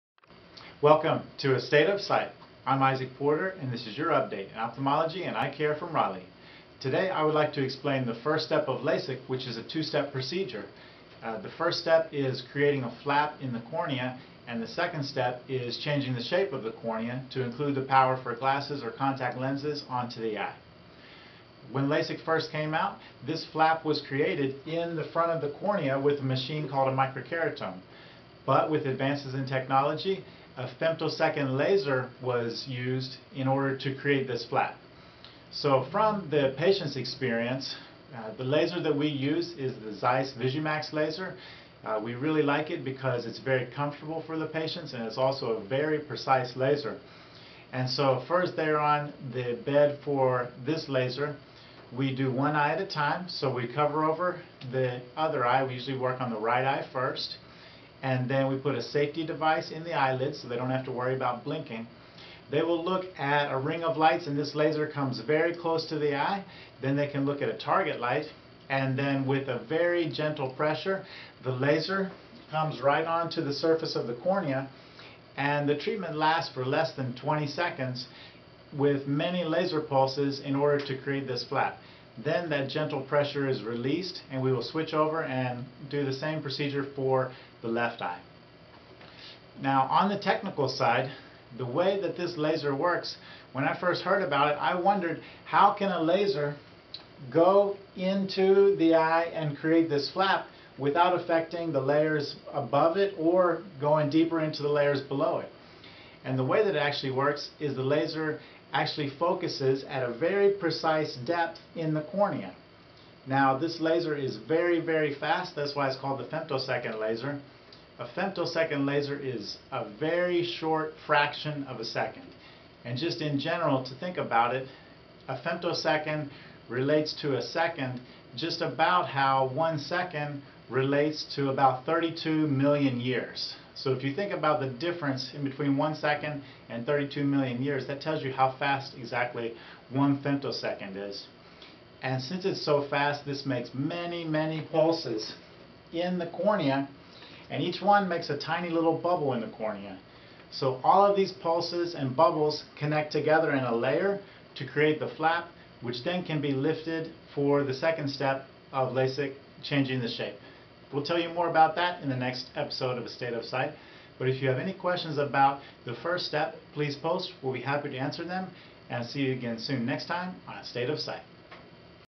LASIK Step #1 - Flap creation with femtosecond laser - A State of Sight #130
LASIK is a two step laser vision correction procedure to improve vision. The first step is creating a flap in the cornea in the front of the eye.

Watch this episode of A State of Sight with Isaac Porter, MD to learn more about the first part of LASIK eye surgery.